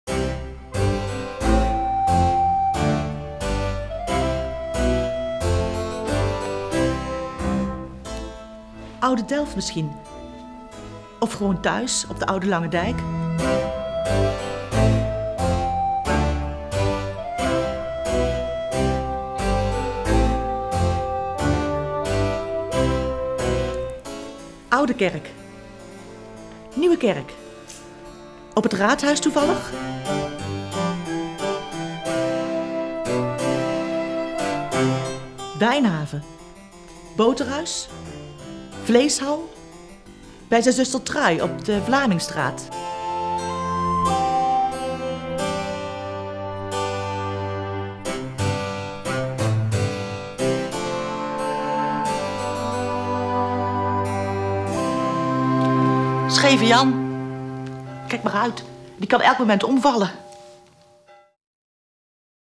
App Waar is Vermeer
Een wandeling door het Delft van 1665 met Ariaentgen Claes van der Minne.